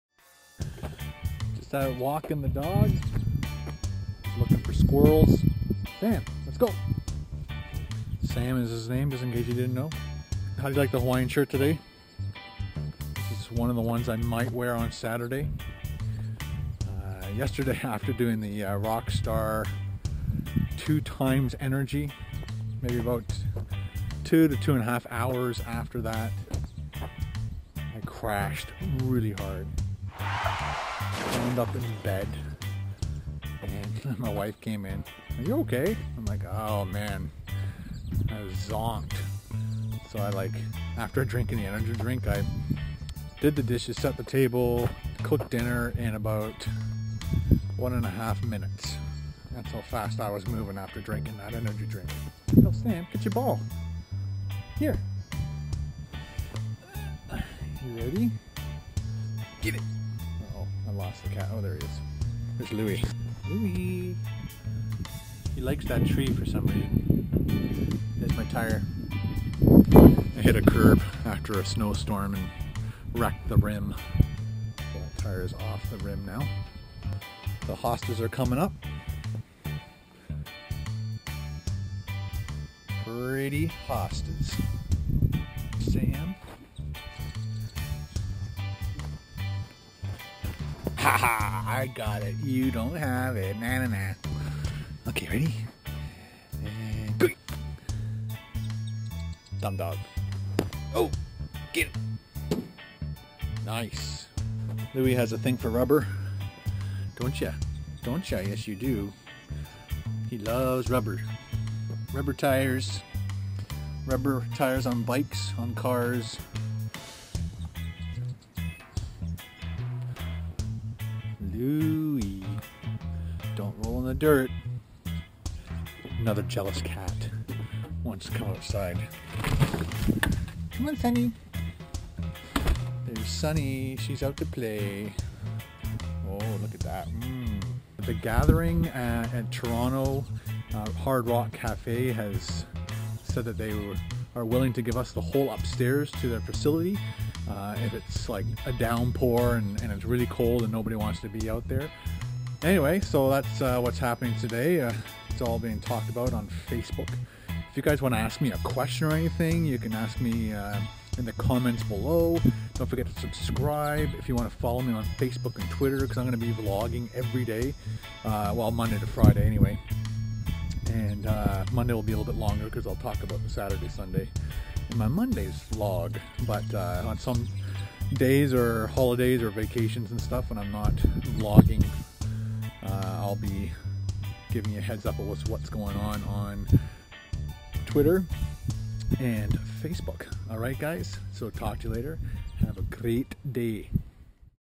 Ken'sVlog 004 - Rockstar Hangover, Dogs & Cats and The Gathering update :)
Rockstar Hangover, Dogs & Cats and The Gathering update :)

These vlogs are just a day by day video diary about what is going on. I even address questions given to me.

Ken Domik
KBDProductionsTV

ISRC: US-UAN-11-00808